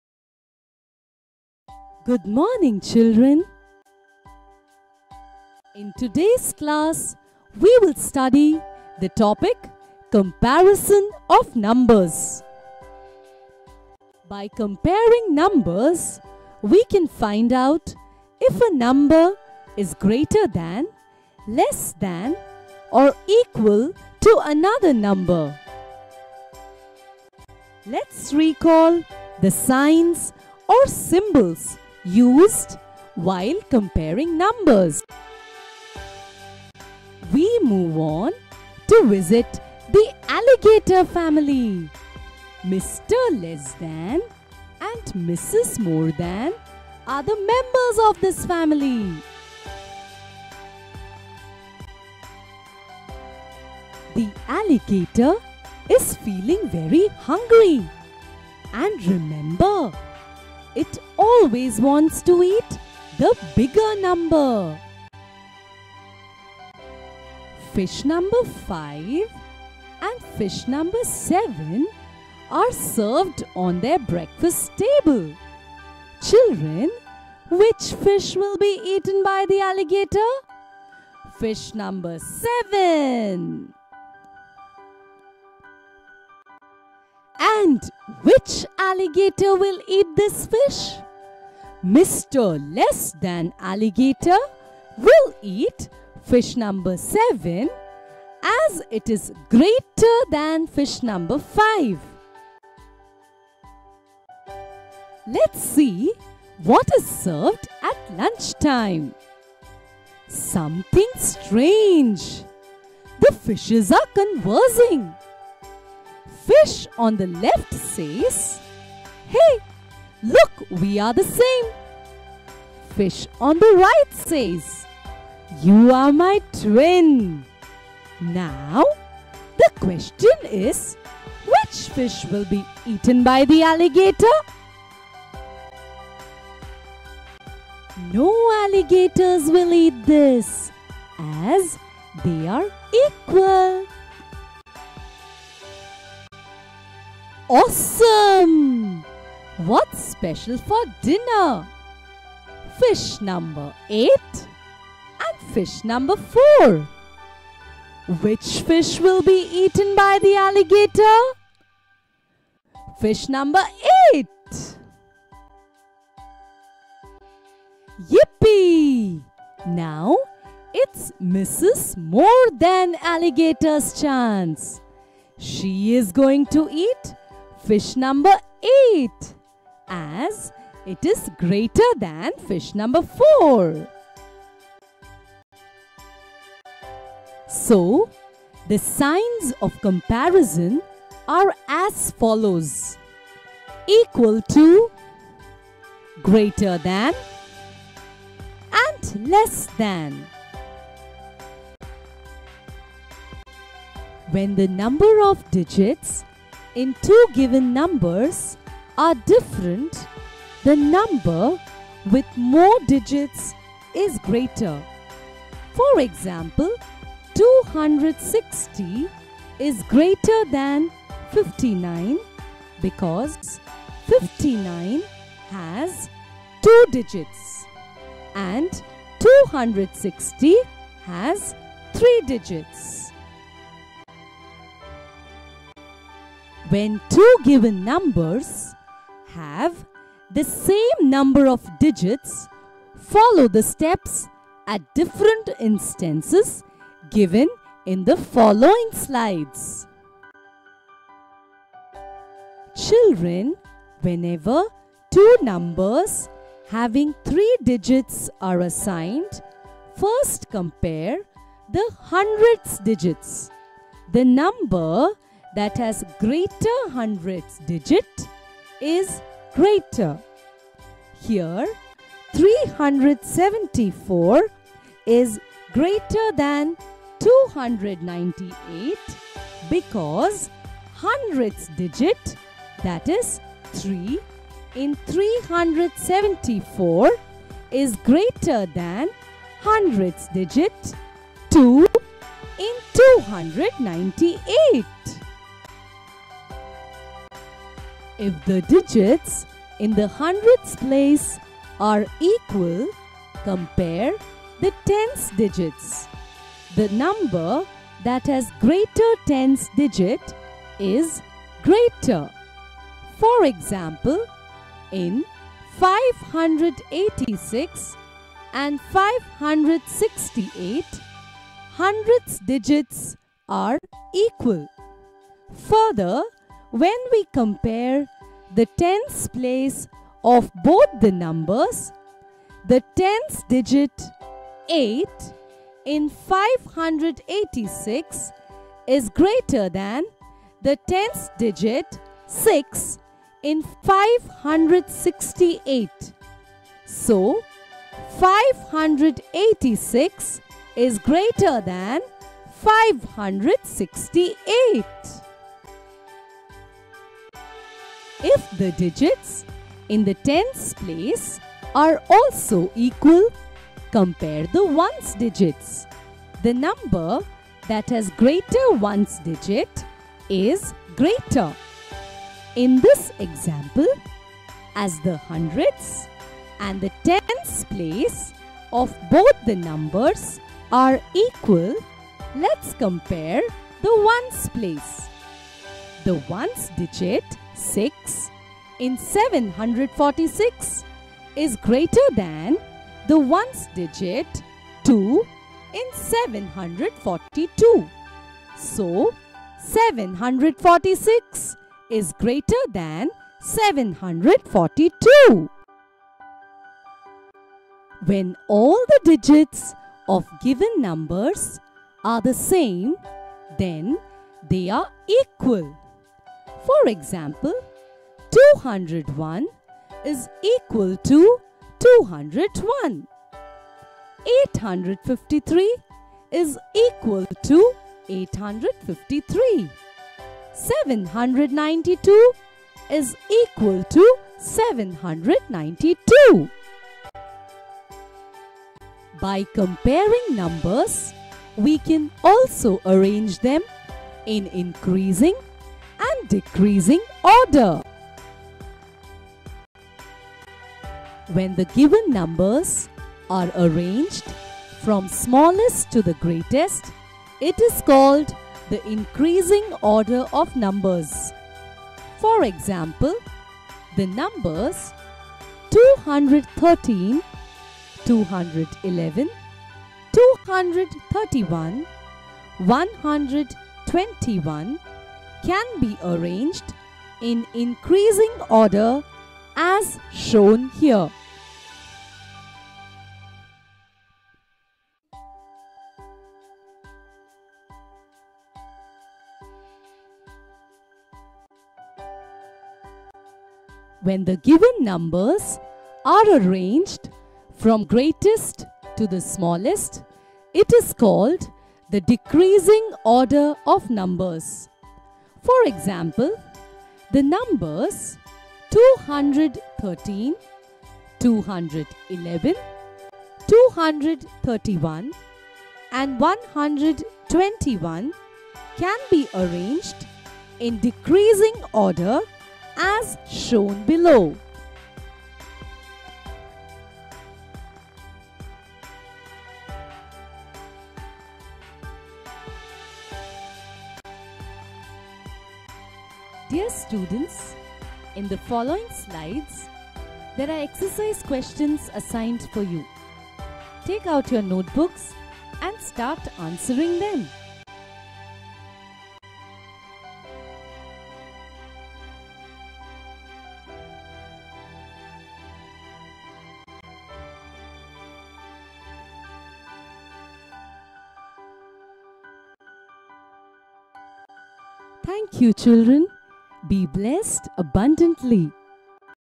COMPARISON OF NUMBERS - GRADE 2 - By JEENA LIJO
This is a video lesson for Grade 2 students to understand the importance of comparing numbers, signs or symbols of comparison, steps of comparing three-digit numbers and arranging numbers in increasing and decreasing numbers.
This lesson contains illustrations, explanations and exercises based on the topic with simple and interactive- narrative explanation.

Prepared by: Jeena Susan Lijo
Softwares used: MS Powerpoint